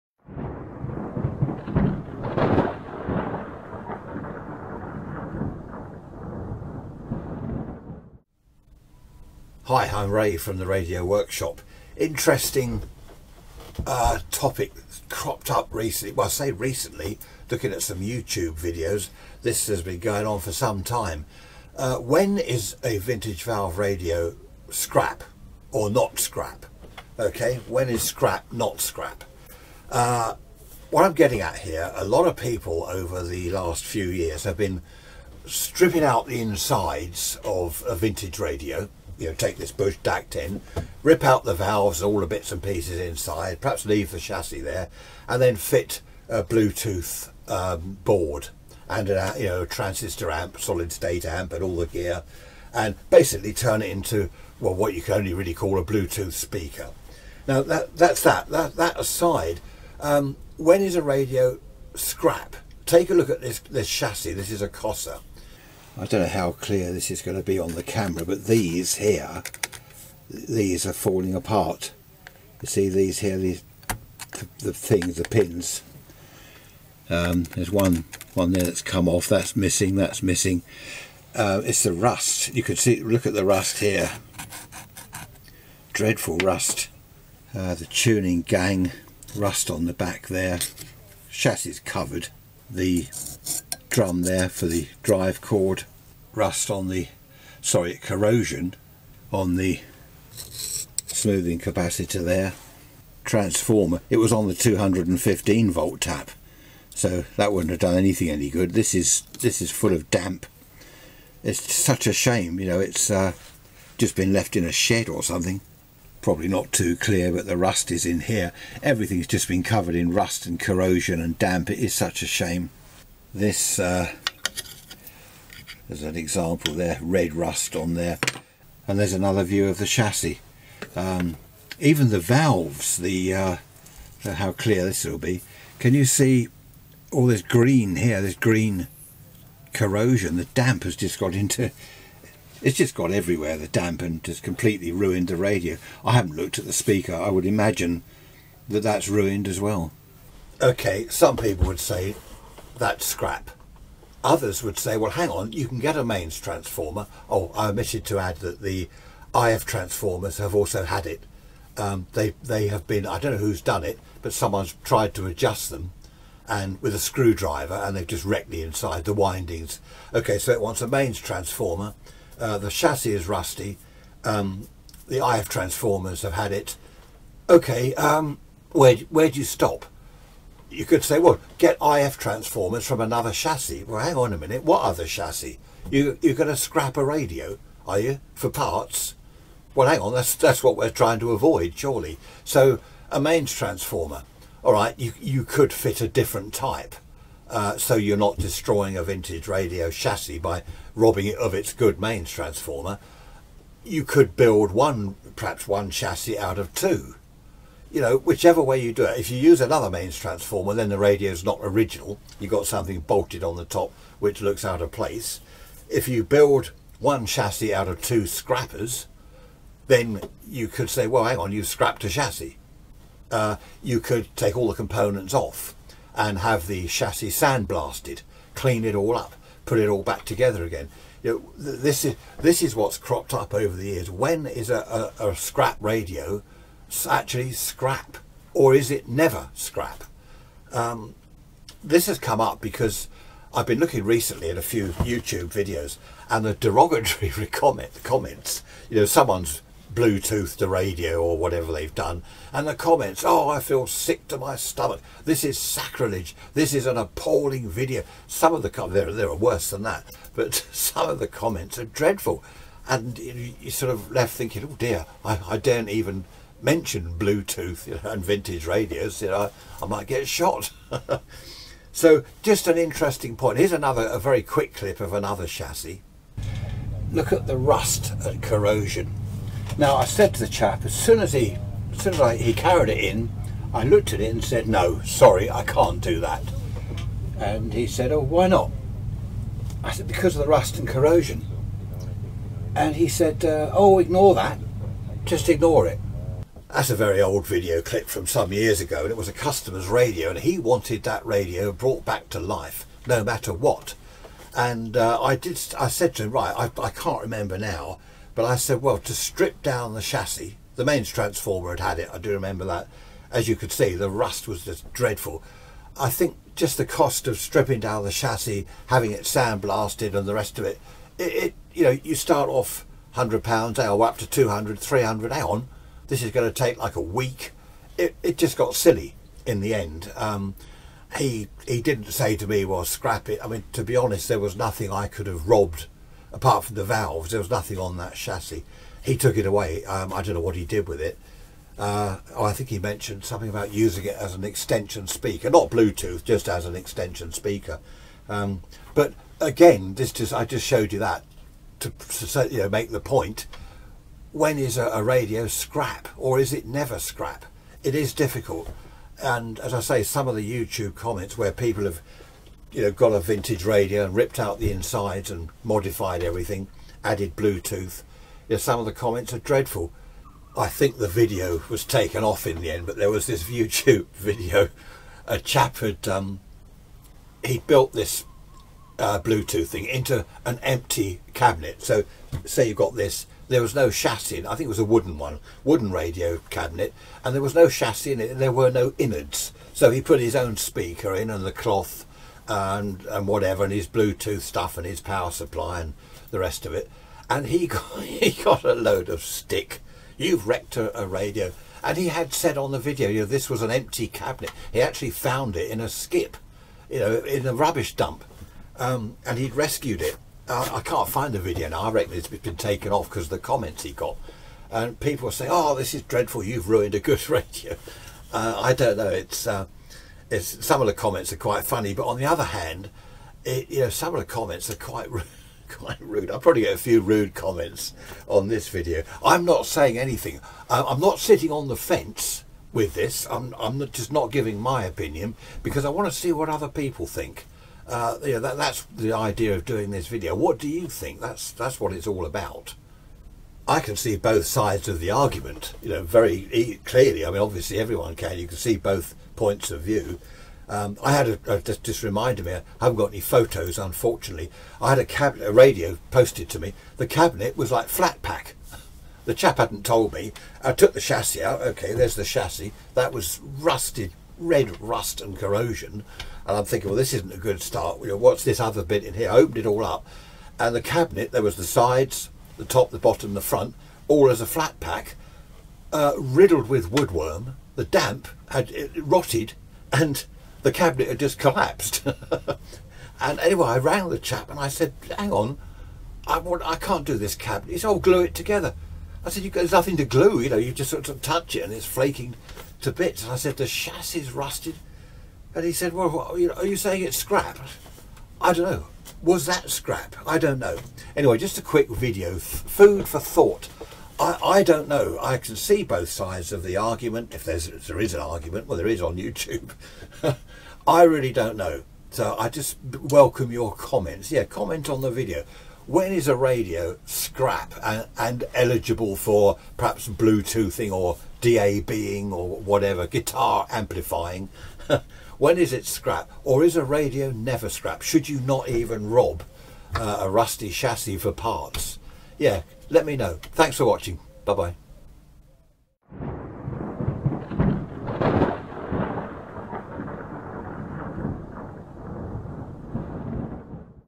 Vintage valve radio scrap Bluetooth DAB conversion
When can a vintage valve radio be classed as scrap and eligible for Bluetooth DAB etc conversion?